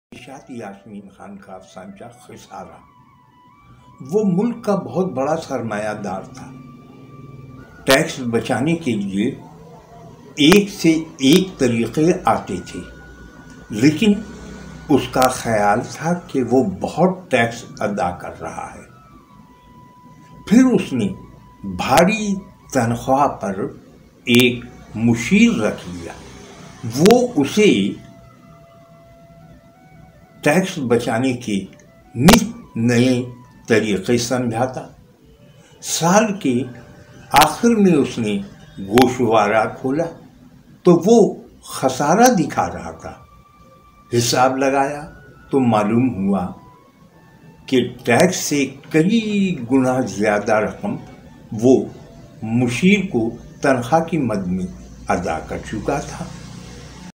nishat yasmin khan ,khasara
in this video ,i recite a shortest story of Nishat yasmin khan.
اس ویڈیو میں میں آپ کو نشاط یاسمین  خان کا افسانچہ خسارہ سنا رہا ہوں